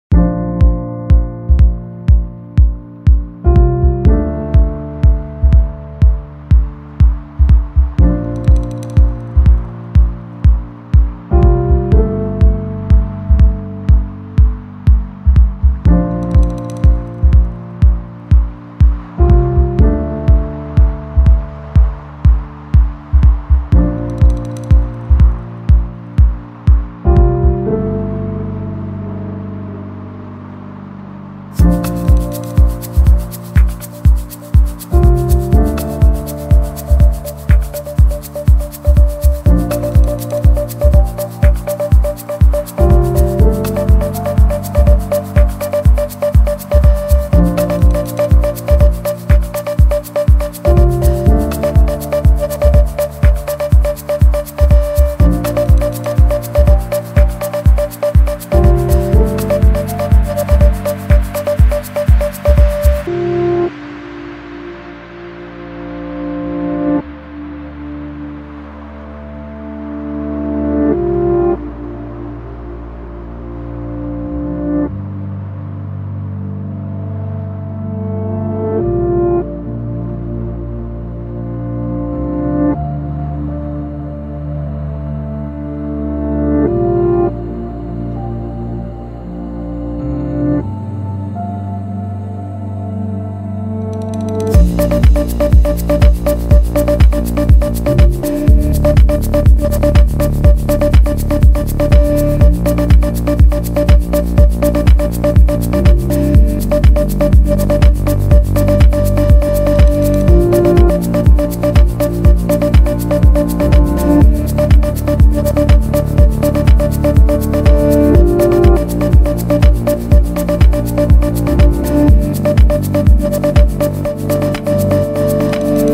Painting a 2k+ Sq ft, (4 story) mural in 8 days
I knocked out this 4 story mural I'm the heart of wynwood in just over a week. shouts to drone exelbierd , kobra paint, ltc equipment... and Florida international university for helping to make it happen.